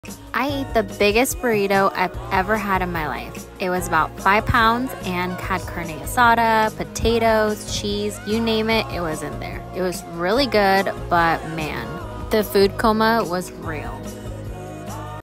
The BIGGEST burrito!
We at the BIGGEST burrito we've ever seen!

Connect with Michelle & Stephen!

Michelle & Stephen are a couple who do food, travel, and date night videos and vlogs.